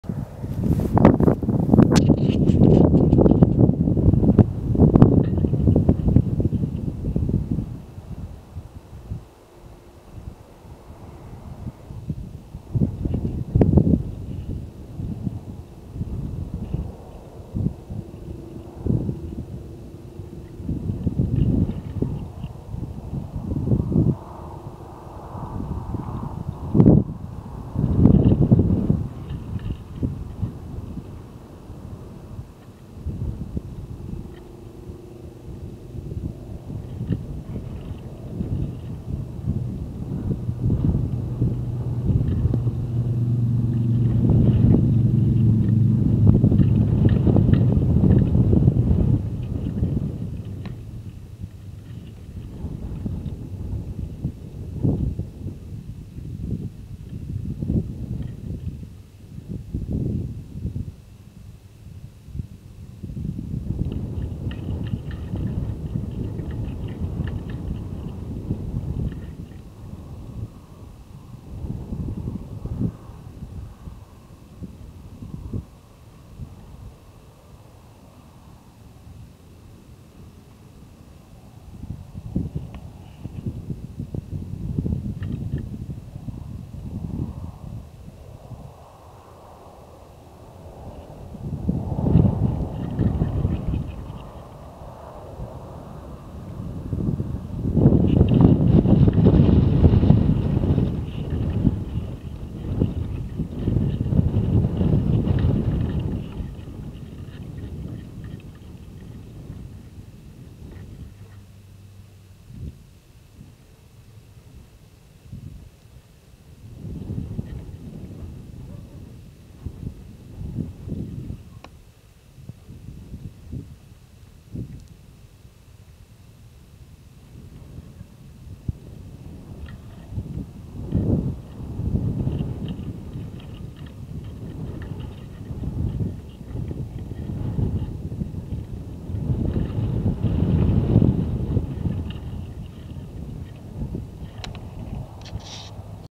Halstead Fire 8/5/2012
Column on SE Flank of the Halstead fire near Stanley, Idaho.
Fire was cresting ridge above Cape Horn Guard Station.